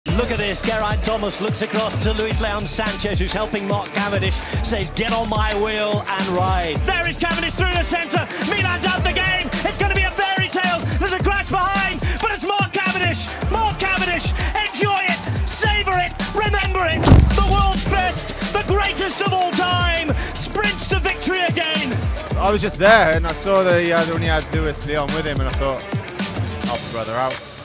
Geraint Thomas 🤝 Mark Cavendish
An emotional moment to finish off the 2023 Giro d’Italia as Geraint Thomas leads out his former teammate Mark Cavendish to the win in Rome 🥹

Never miss a moment with the best racing coverage on GCN+ 🤩 To find out more, check out gcn.eu/unmissable